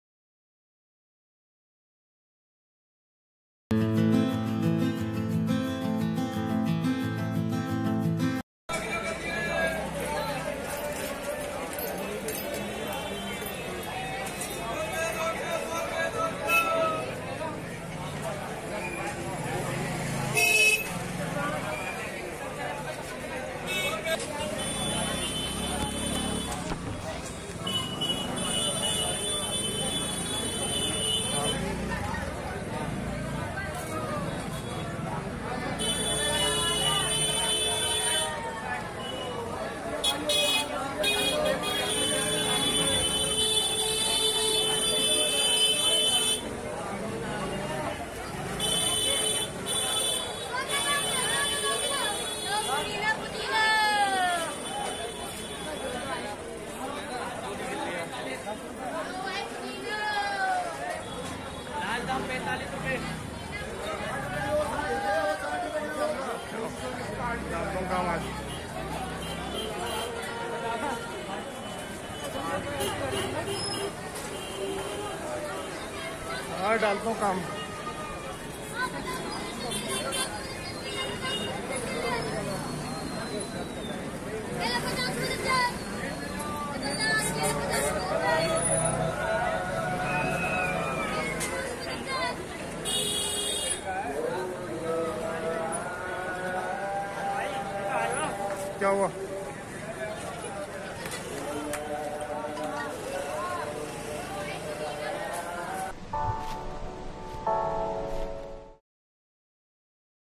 Chitli qaber mein lockdown me logo ki bhed kharidari karte hue Delhi
PLEASE